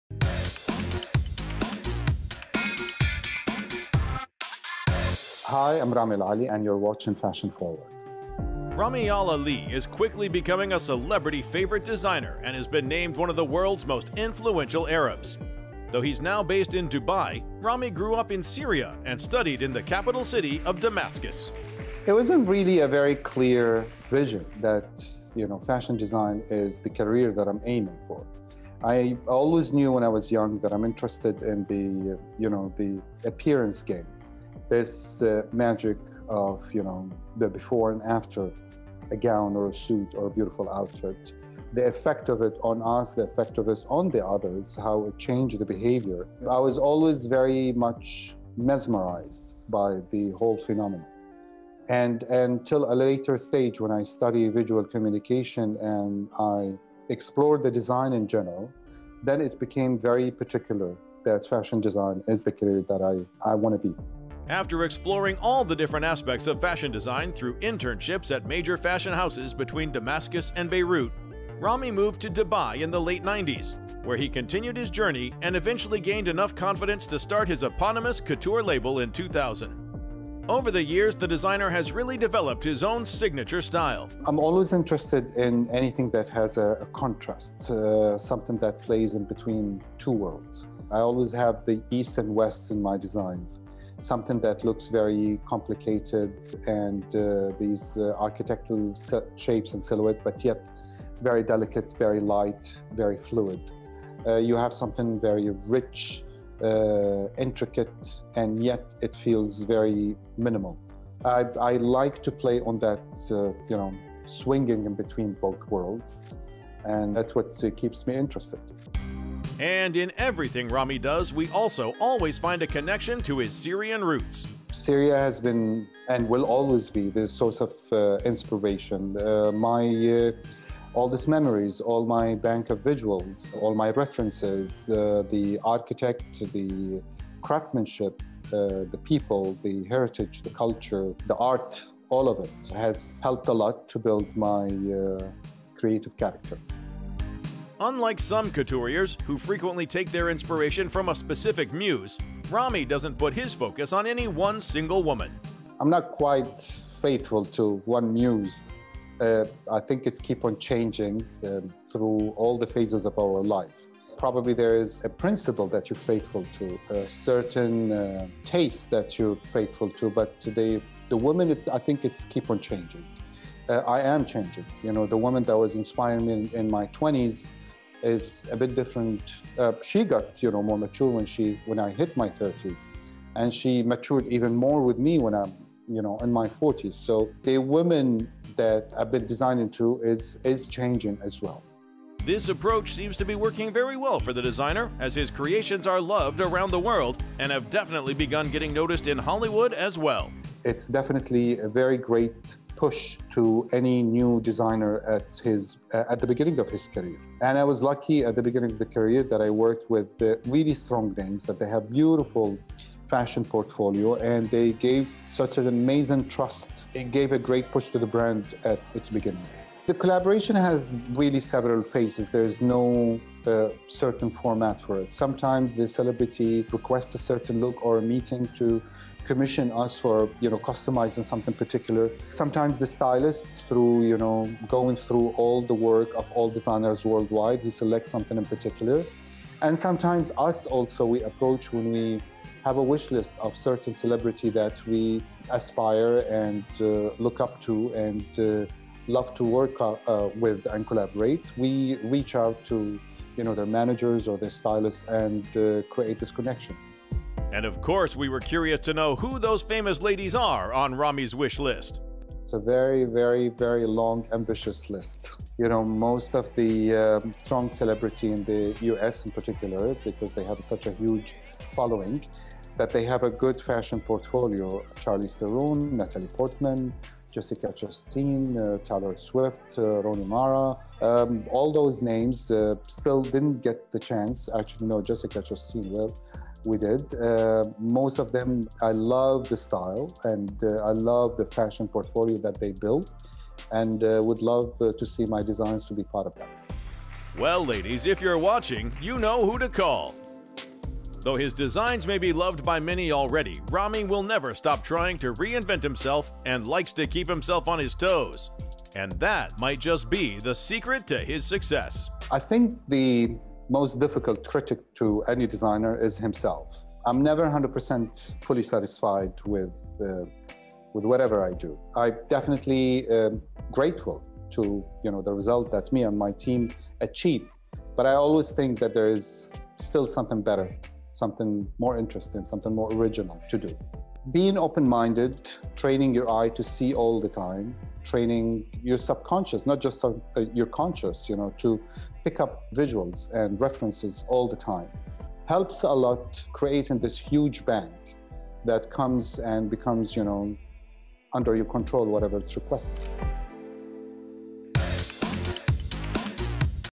Fashion Forward Fashion Rami Al Ali
Couturier Rami Al Ali interviewed by Fashion Forward regarding his 20 years  journey as a fashion designer